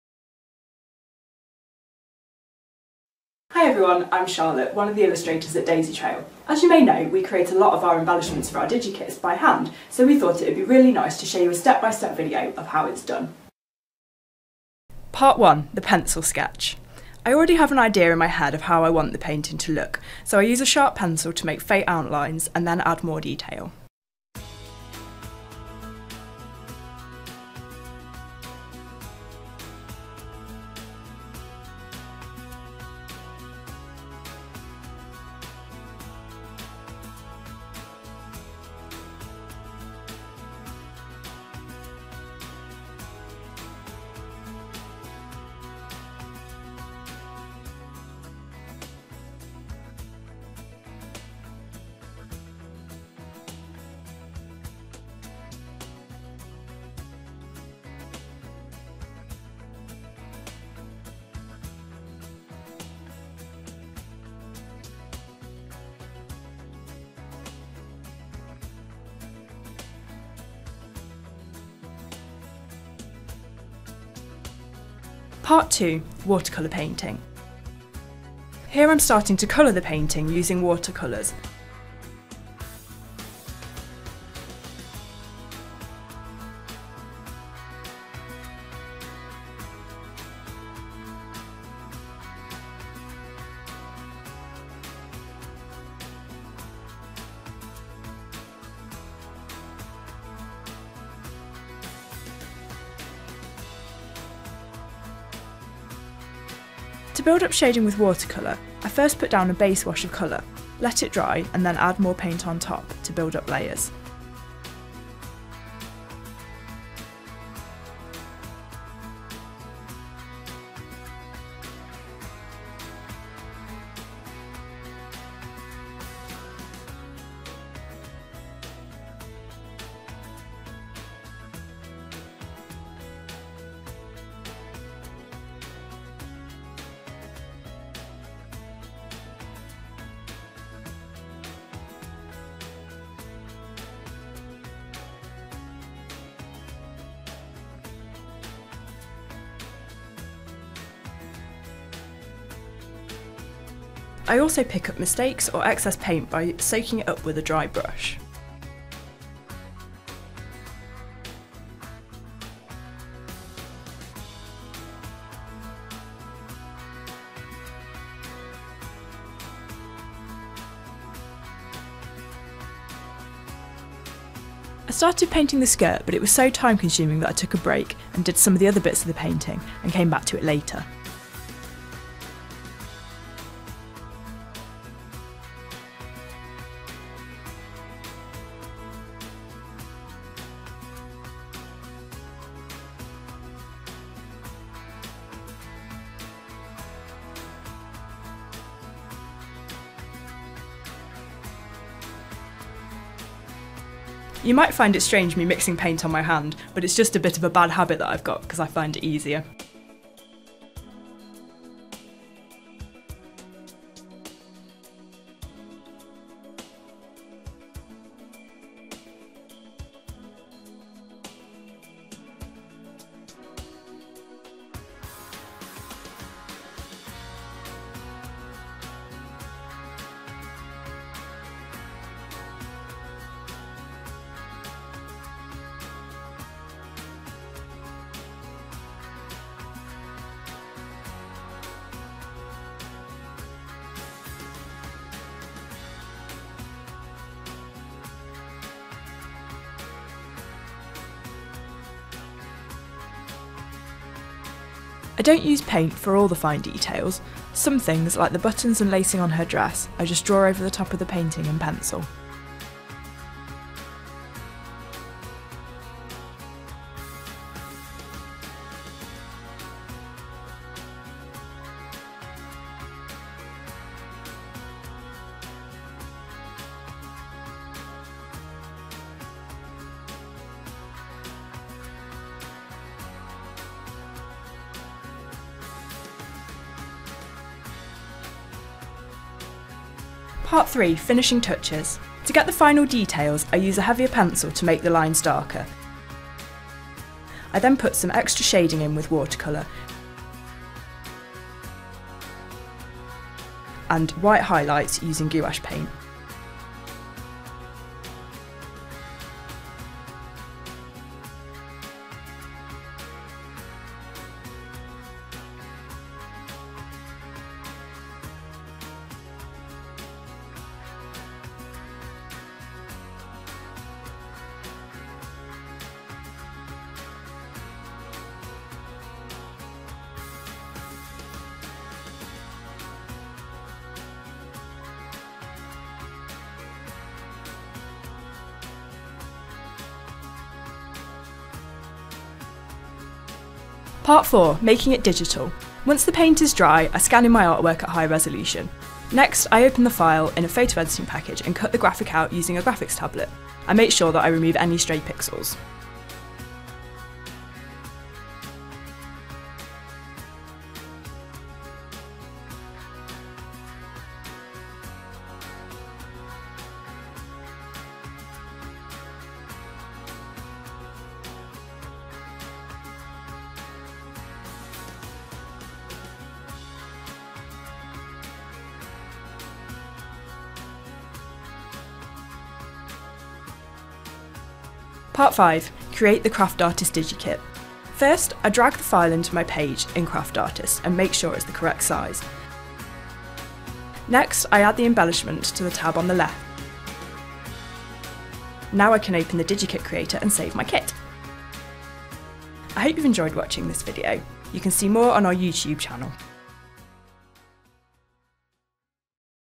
A Day in the Life of a DaisyTrail illustrator
Here at DaisyTrail we have a couple of incredibly talented illustrators, who hand-draw a lot of the characters for the DaisyTrail digikits. In this video, we show you how Charlotte creates one of the characters in the Traditional Christmas digikit She starts with a pencil drawing, then adds colour with watercolour paints before converting her drawing to a digital file and saving it in a CraftArtist kit. We hope you like it!